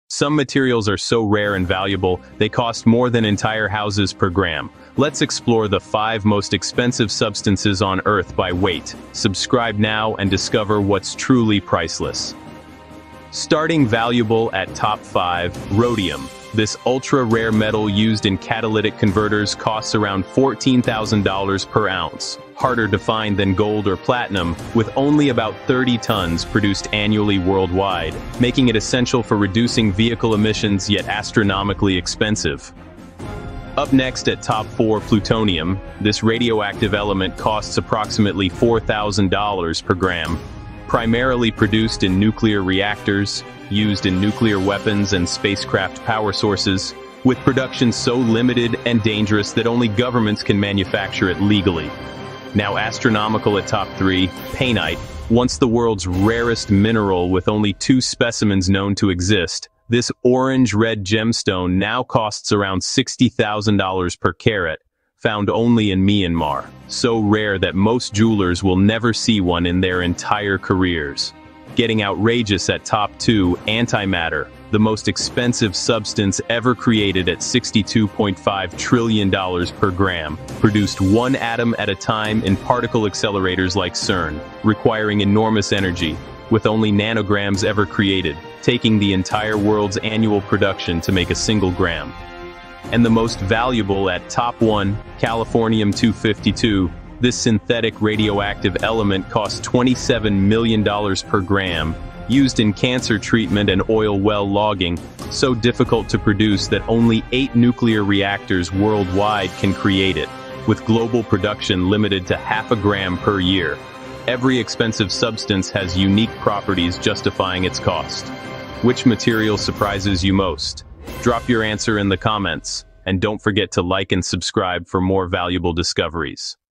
5 MOST EXPENSIVE Substances 💎
From antimatter to rare crystals, these are the most expensive substances on Earth by weight! 🤑
Discover what makes these materials so rare, valuable, and fascinating.

most expensive substances, rare materials, valuable elements, precious materials, science facts, rare crystals